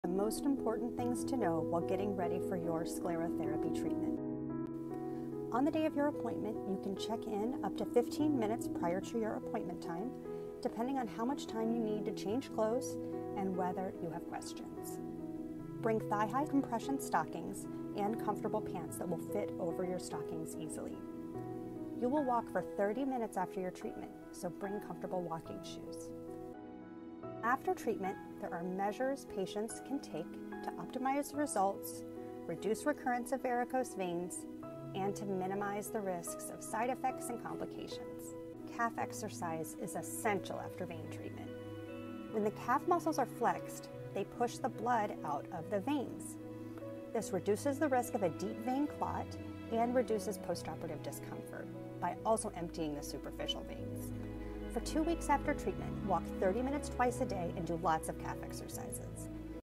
The Science of Foam Sclerotherapy: Your Guide to Treatment and Recovery
Dr. Sarah Lucas, vascular surgeon at La Jolla Vein & Vascular explains what to expect and after care for foam sclerotherapy treatment of varicose veins and venous reflux disease.